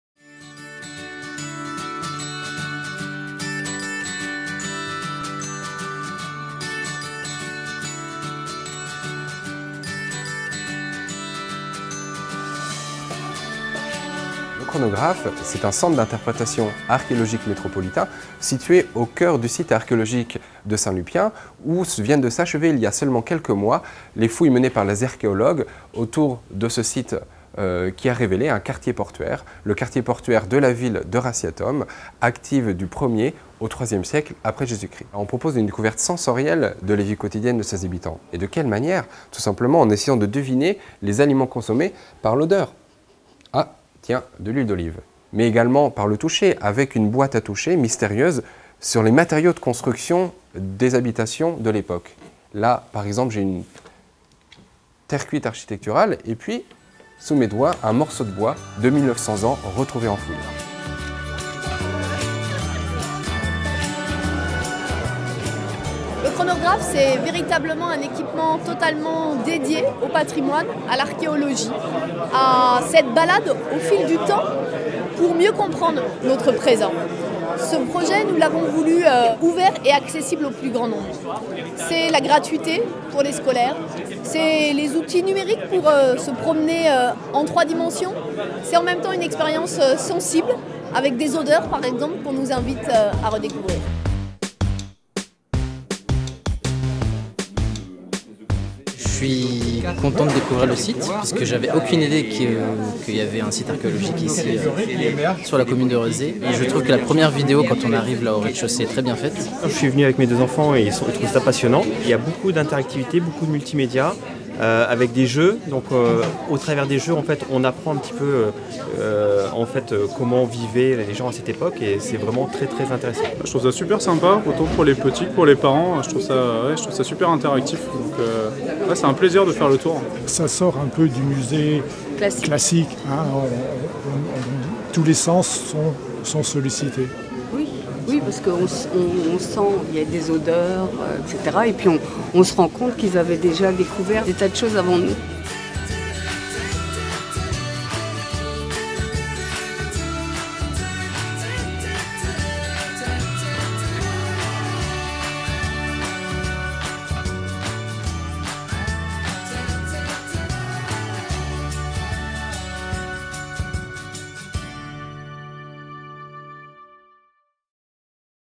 Expérimenter l'archéologie - Le Chronographe
L’histoire de Ratiatum, ville romaine, s’est dévoilée au fur et à mesure des fouilles menées par les archéologues à Rezé depuis les années 80 (Loire-Atlantique, 44).
Des pièces de monnaie jusqu’aux quais portuaires en chêne, en passant par des statuettes en terre blanche, le Chronographe est le témoin de cette histoire et de ces découvertes.

Mettez-vous dans la peau d’un archéologue et faites parler les objets retrouvés en fouille à l’aide des dispositifs numériques, ludiques et interactifs de l’exposition permanente.

Autour du Chronographe, le site archéologique et la chapelle Saint-Lupien prolongent la visite et donnent à voir in situ 2000 ans d’histoire et de traces, laissées par les hommes, de l’Antiquité à nos jours.